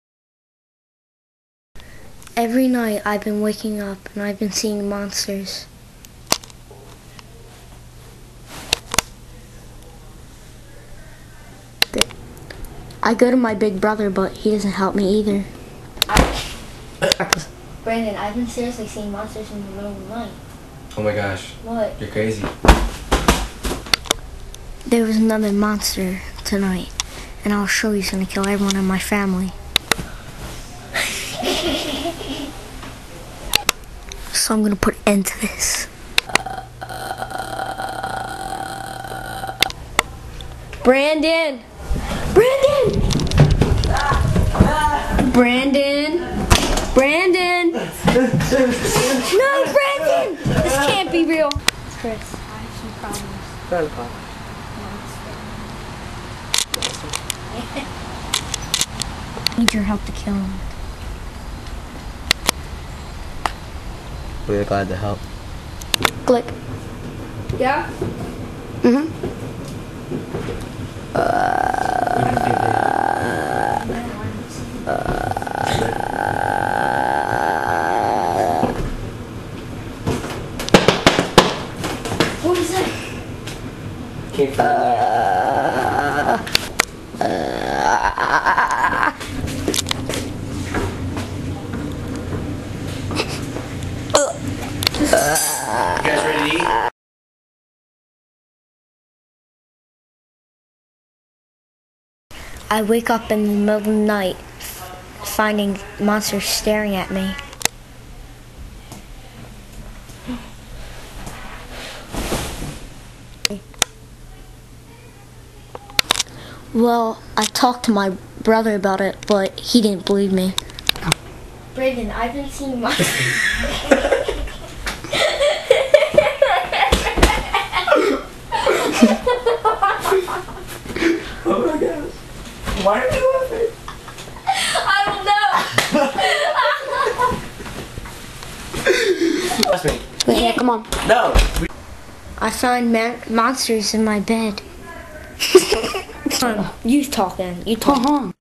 Monsters- officail movie trailer fanmade
a kid who wakes up and sees monsters